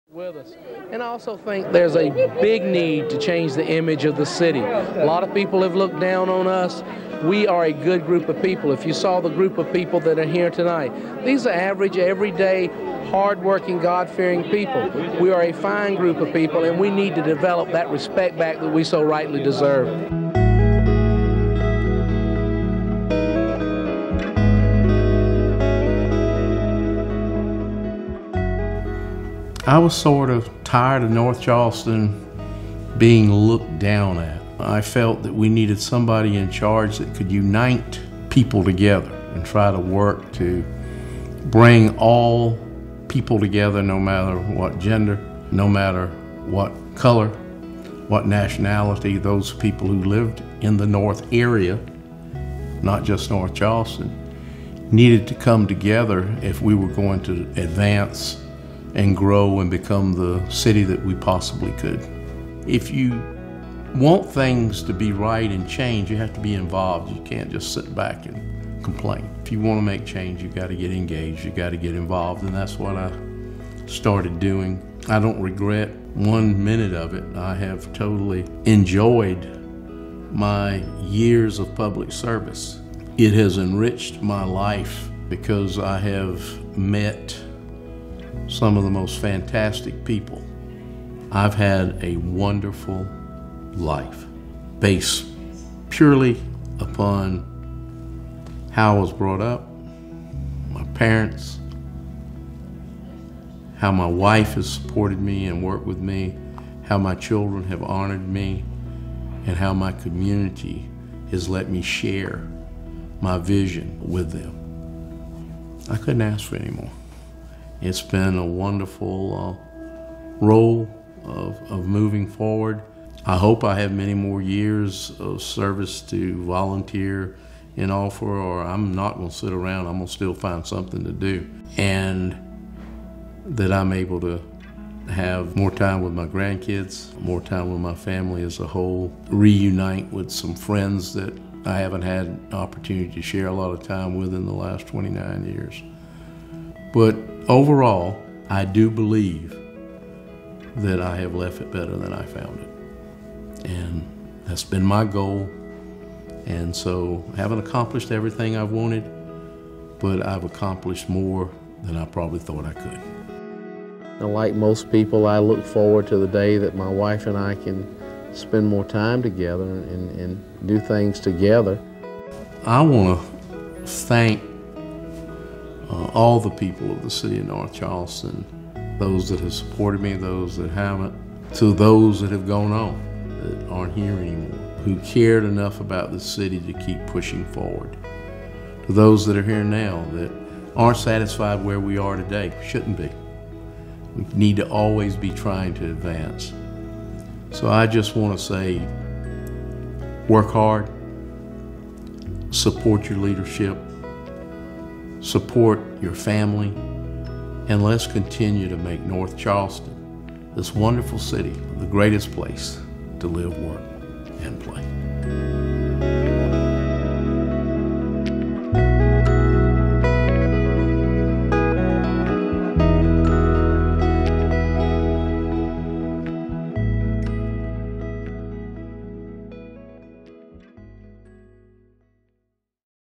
Mayor Summey Reflects on a Journey of Service
Mayor Keith Summey reminisces on the privilege and responsibility of leading North Charleston over the last three decades; a story of community, progress, and gratitude.